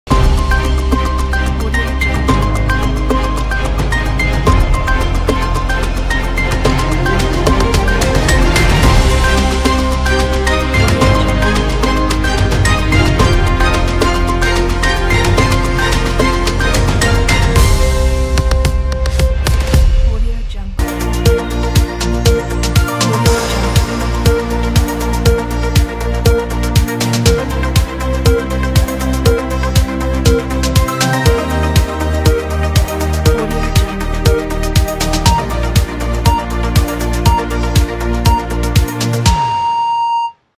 News Headlines Intro Video - After Effects Template
After Effects Template Features:
- Ultra HD (3840×2160)
- No Plugins
- 1 Opener, 1 Quote, 2 Lower Third, 2 Label, 2 Background
- Color Control
- Very Easy Customize
- Fast rendering